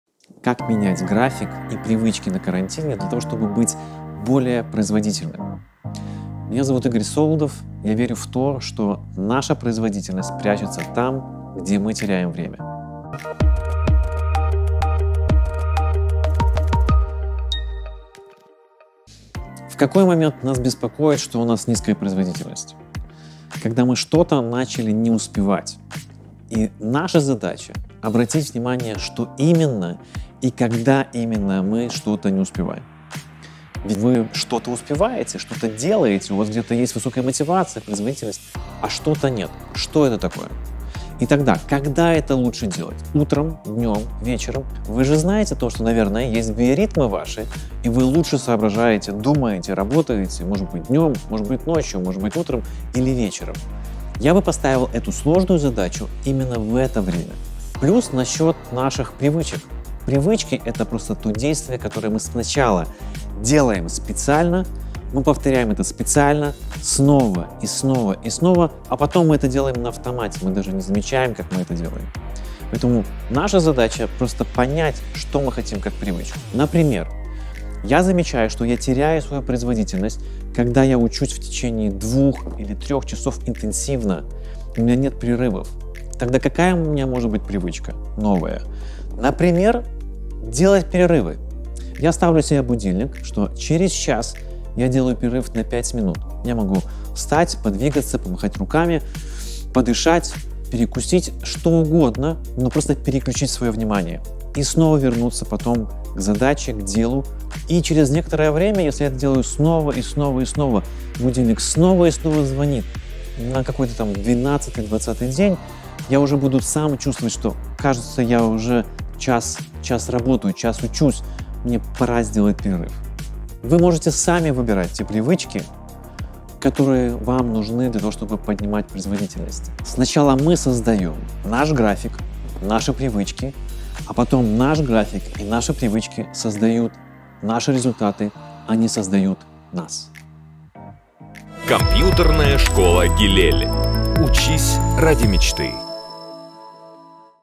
Как быть производительным на карантине
Как менять график и привычки на карантине,  чтобы быть более продуктивными?
Рассказывает Игорь Солодов, коуч лидеров и тренеров.